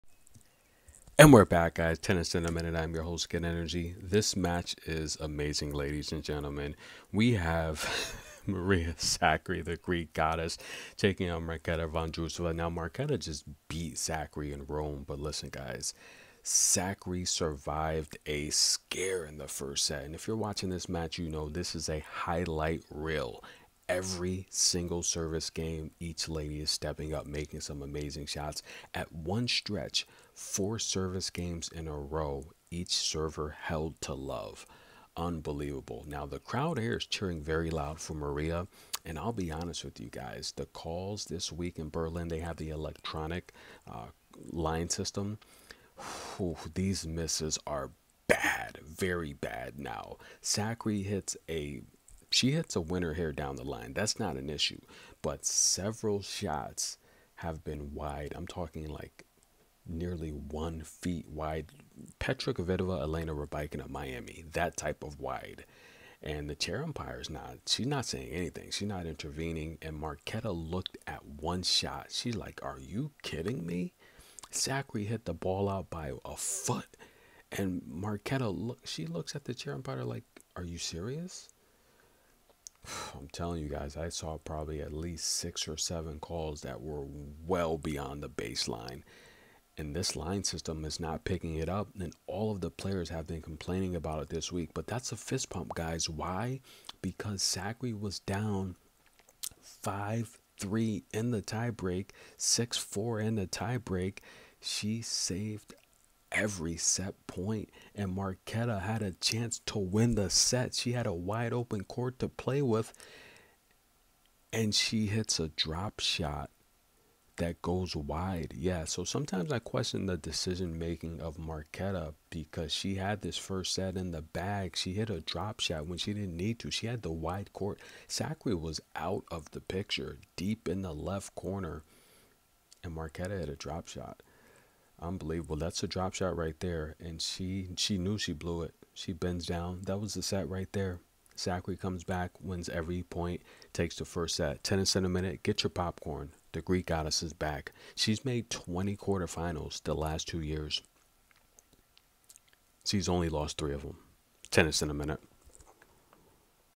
Maria Sakkari survives a scare Vs Marketa Vondrousova Live WTA Tennis Berlin QF
Marketa blows 3 set points